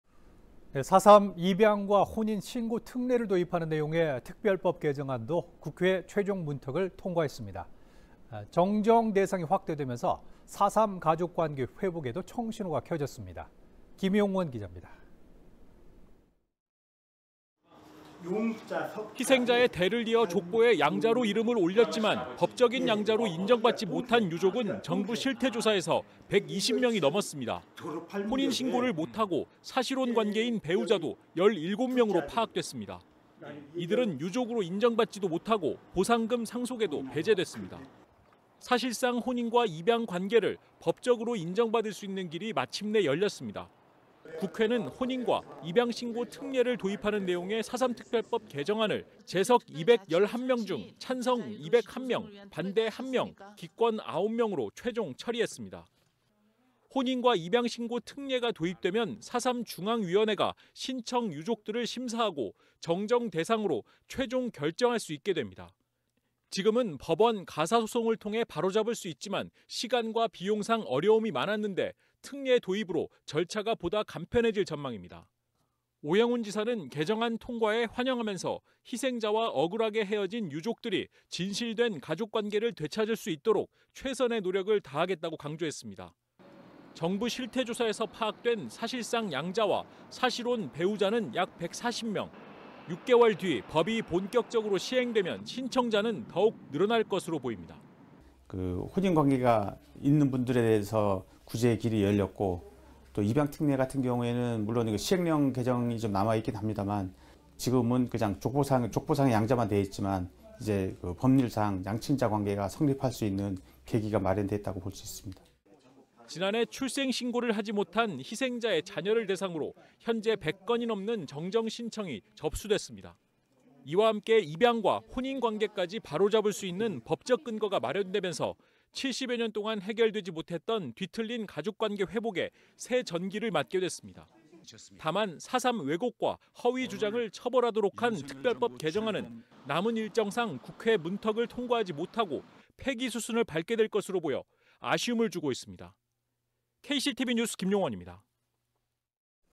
4·3 입양 - 사실혼 '바로 잡는다'…법안 통과
4.3 입양과 혼인 신고 특례를 도입하는 내용의 특별법 개정안이 국회 최종 문턱을 통과했습니다.

정정 대상이 확대 되면서 4.3 가족관계 회복에도 청신호가 켜졌습니다.